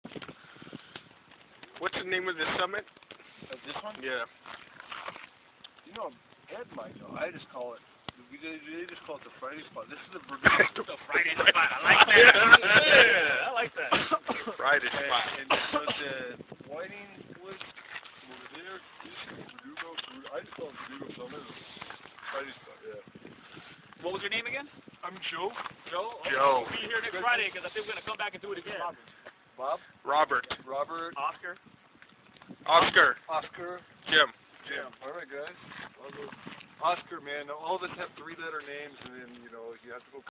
Verdugo Summit Viewpoint
Hiked to the top of Verdugo Summit and hungout to enjoy the night light view.